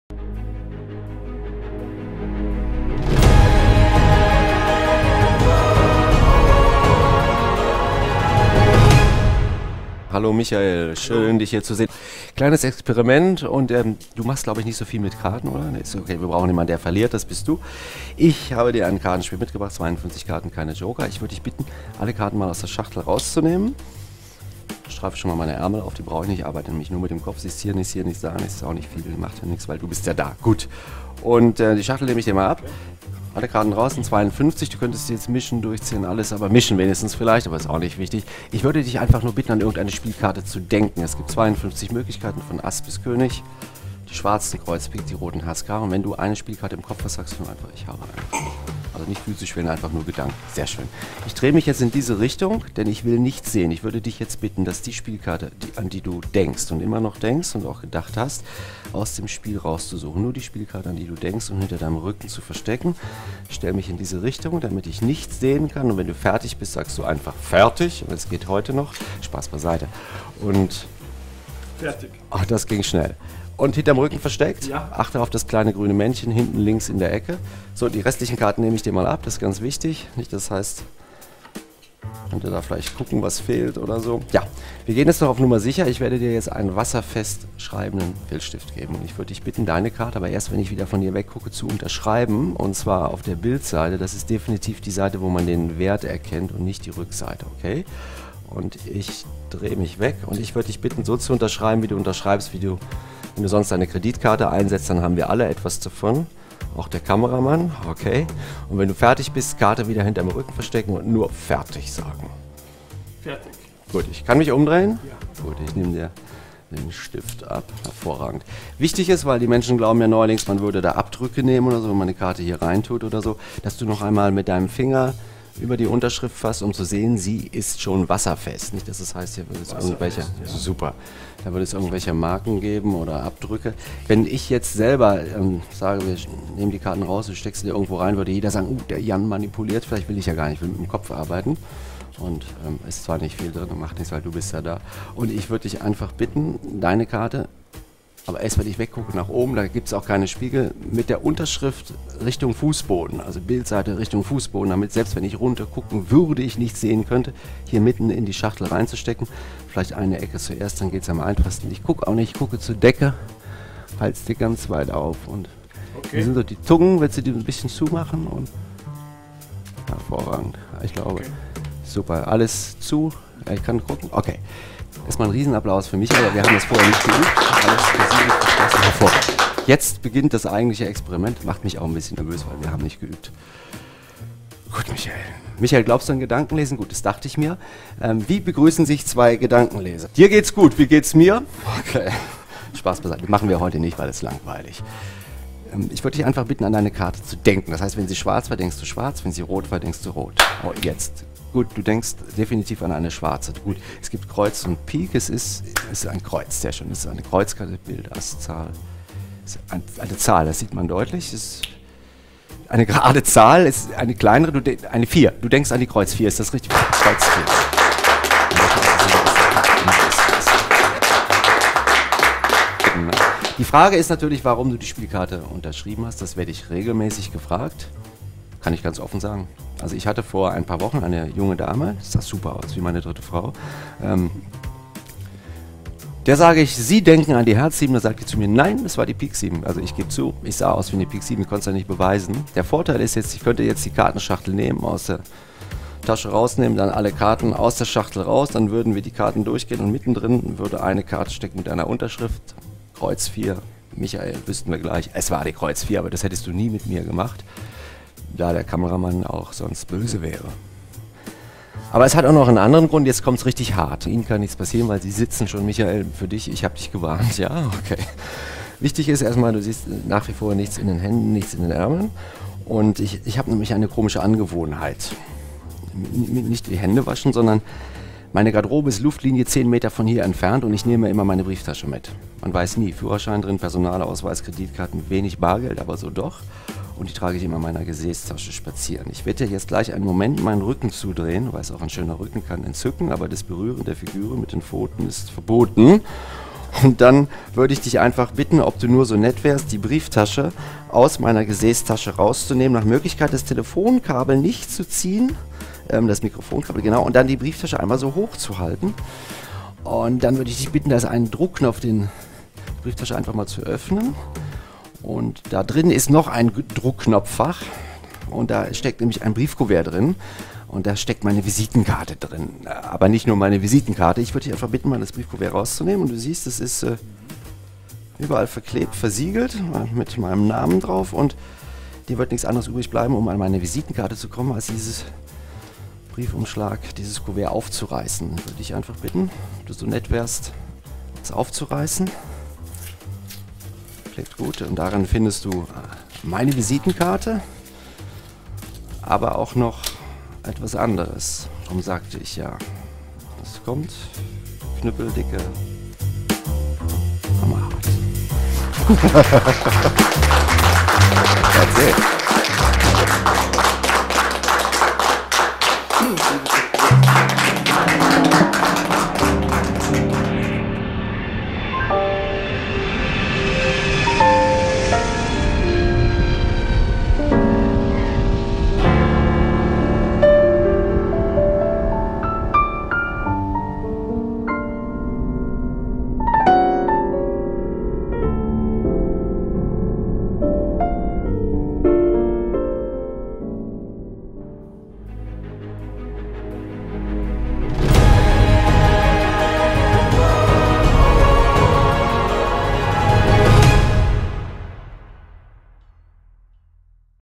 Jan Forster "Extreme Impossible Card To Wallet" - Trailer
Eine

7 Jahre lang entwickelte
7 Jahre lang immer wieder verfeinerte
7 Jahre „under heat" in der Praxis erprobte
7 Jahre lang erfolgreich gehütete
7 Jahre lang weltweit einmalige

Routine mit 2! das Publikum erschlagenden Effekten!

Jan Forster sagt selber:

„...eines meiner Lieblingskinder ist erwachsen geworden..."

„...Ich hätte nie gedacht, dass es Kollegen geben würde, die es schafften, mich so lange zu bearbeiten, bis ich zustimmen würde, diese Routine in einer Mini-Auflage „auf den Markt zu bringen". Immerhin betrachte ich sie als meine stärkste, nicht rein mentale Routine... So kann man sich irren. ."

„...Ich habe eine Reihe von Firmenkunden, die sich explizit immer wieder diese Routine wünschen, sie oft gesehen und nach wie vor nicht die geringste Erklärung dafür haben. ..."

Nur 25! Stück für den Weltmarkt!

Exklusiv erhältlich bei Mental-Treasures*

* Mental Treasures ist eine Marke der ICS Media GmbH